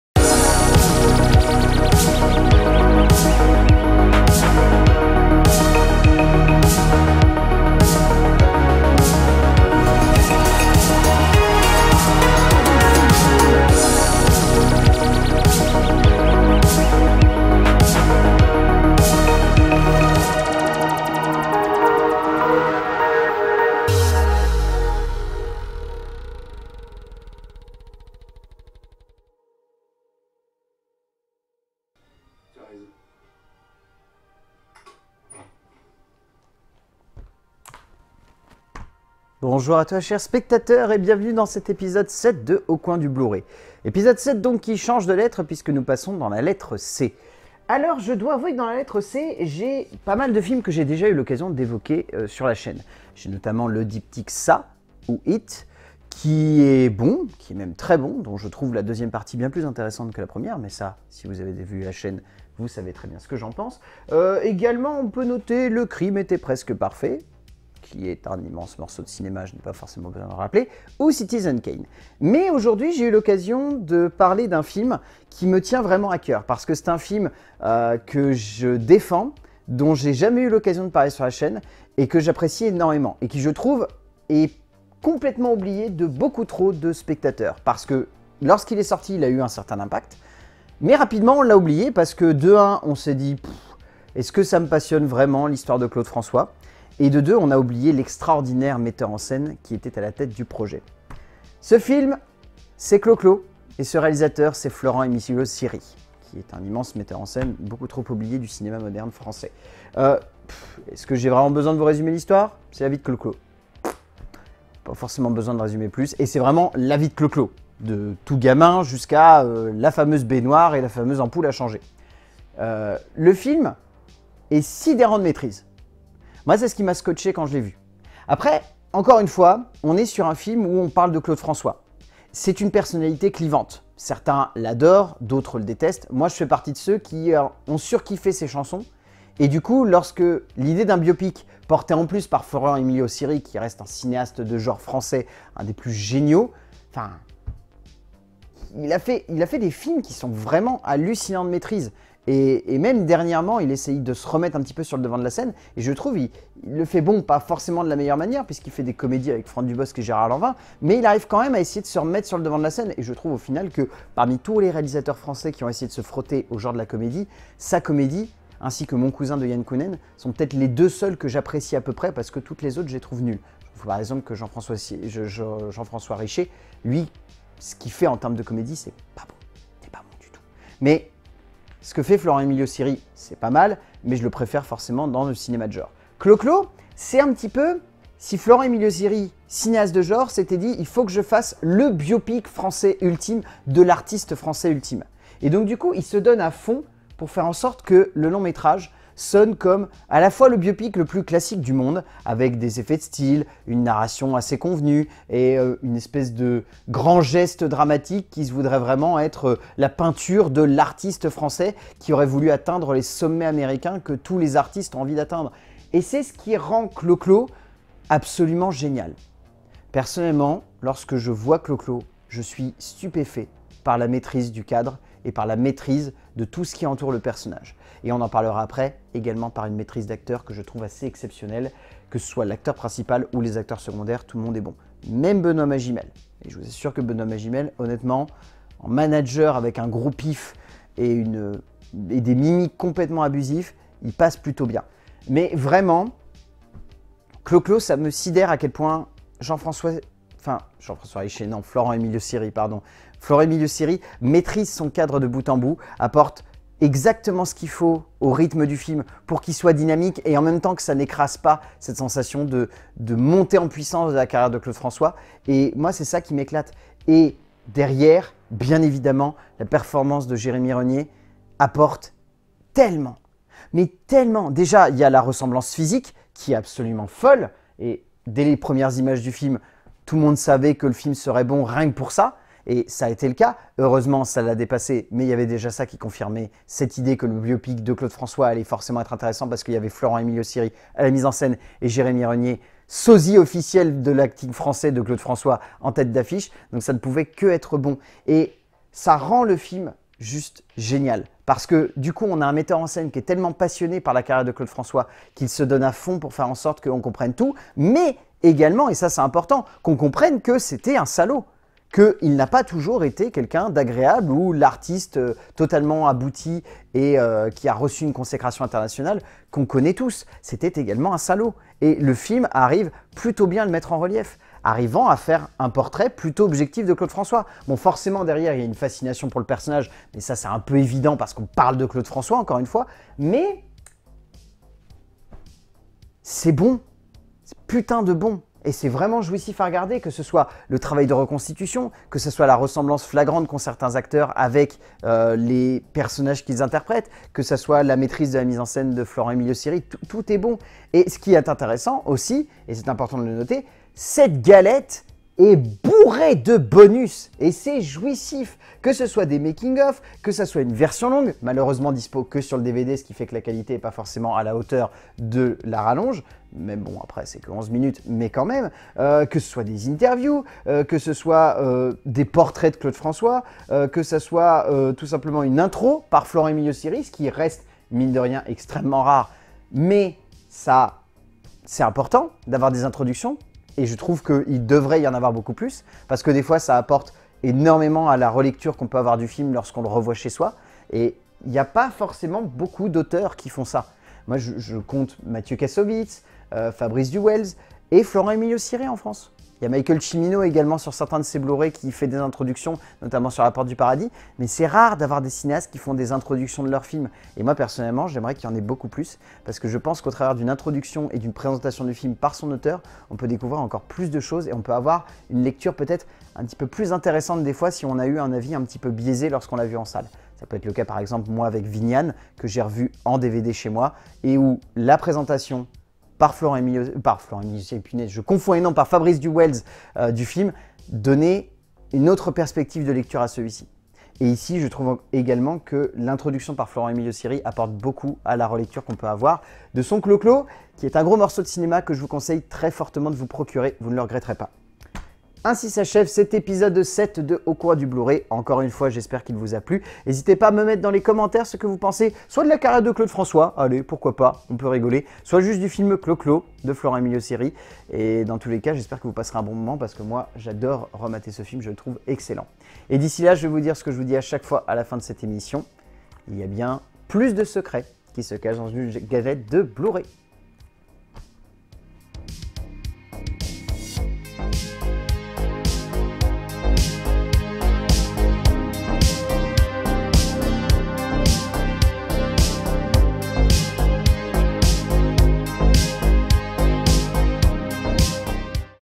Au Coin Du BLU-RAY Ep. 7 - Cloclo
Après une année 2020 riche en émotion, ou la culture était extrêmement importante, nous avons cherché à brasser toute une partie de ma collection de support physique, au travers des DVD, partons dans une autre contrée, celle des Blu Ray.
Cette semaine, je vous emmène au cœur d'un biopic qui a fait sensation à sa sortie, mais qui aujourd'hui, semble complètement être tombé dans l'oubli, "Cloclo"